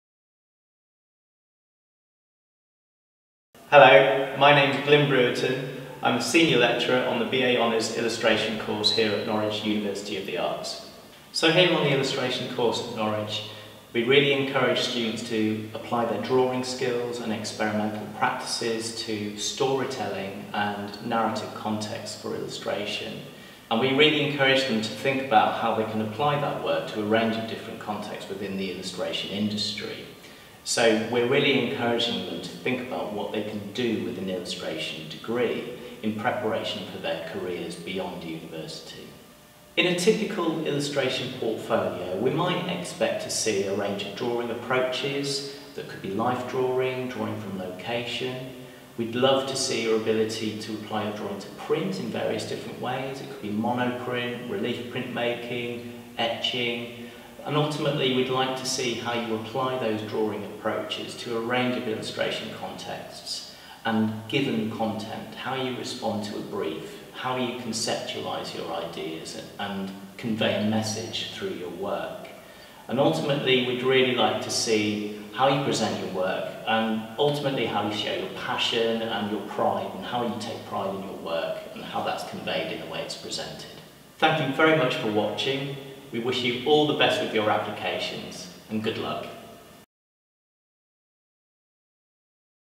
Portfolio tips for BA Illustration | Norwich University of the Arts
When applying to BA (Hons) Illustration at NUA, we want to see a range of drawing approaches and how you respond to a brief.

Get tips from Senior Lecturer Glyn Brewerton on what we're looking for in your portfolio when applying.